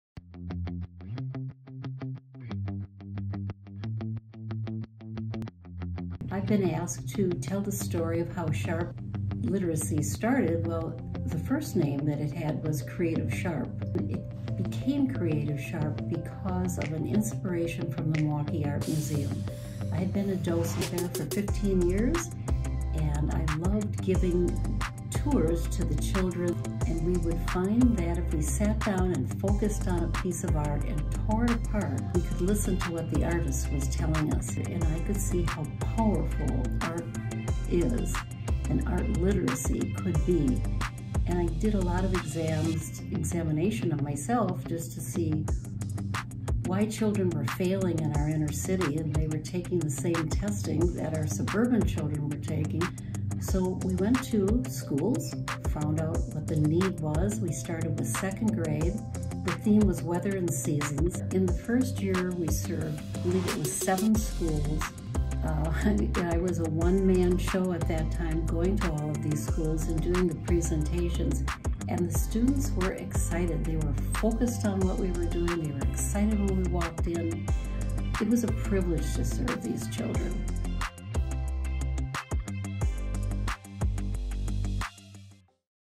Episode 2: In the Beginning
Marlene tells the story of how SHARP Literacy started. Formed originally under the name “Creative SHARP,” the non-profit drew inspiration from Marlene’s time leading educational tours for children at the Milwaukee Art Museum. Marlene emphasizes the importance of focusing on art and extrapolating what the artist is telling us.